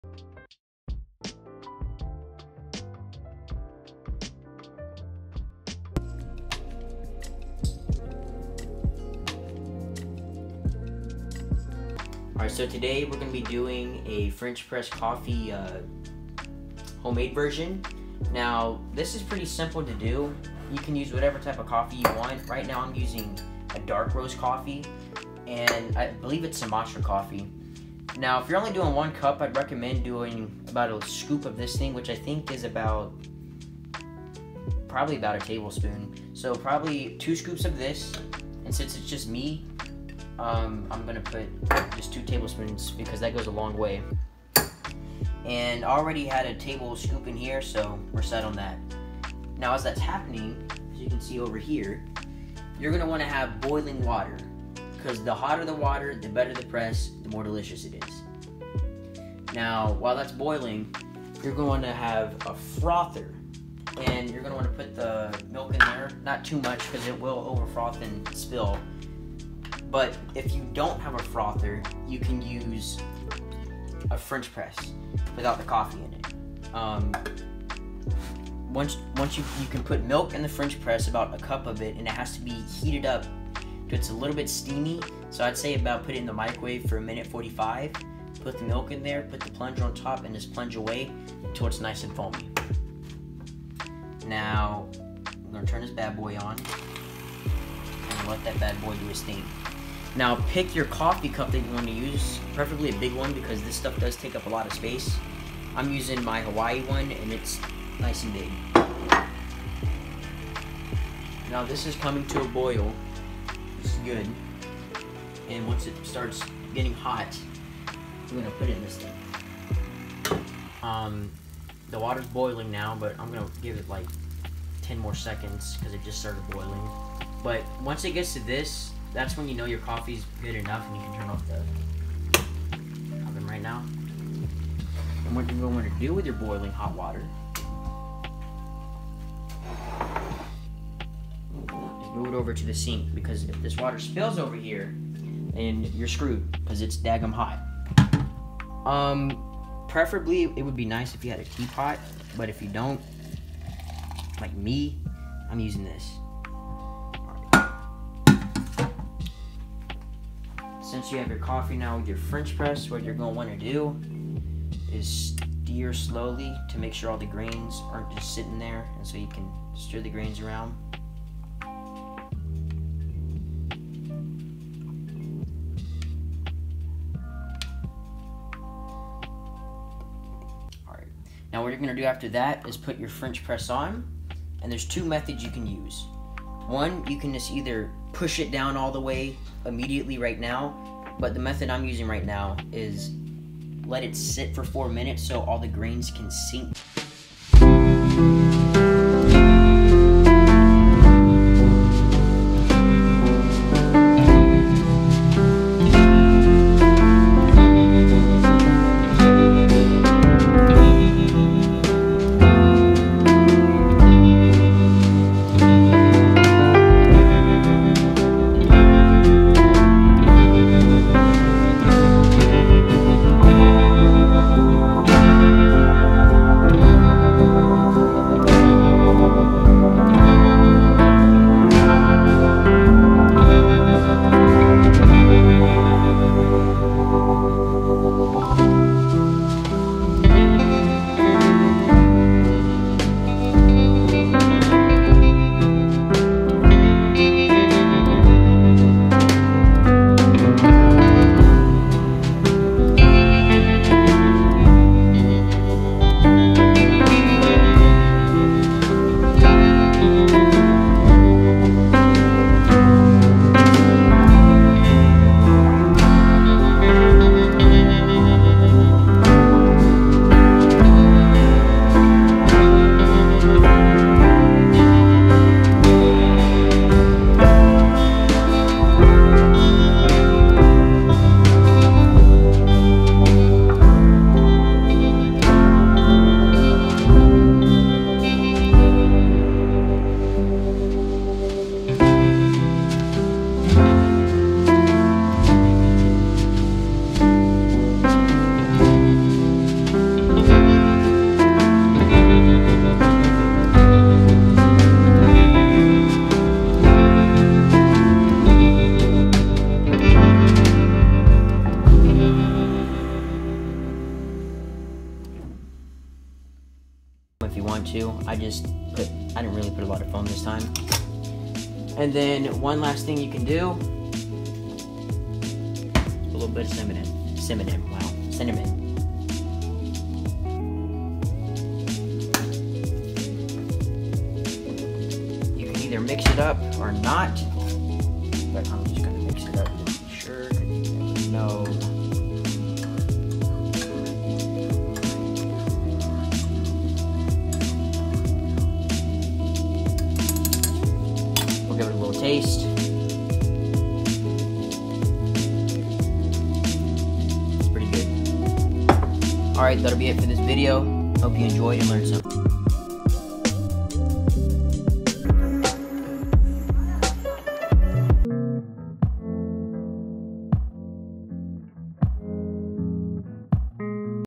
Easy French Press Recipe To Do At Home
This is my first coffee video, so let me know what I can improve on in the next video. As always you should make coffee how YOU like it so feel free to mix and match recipes.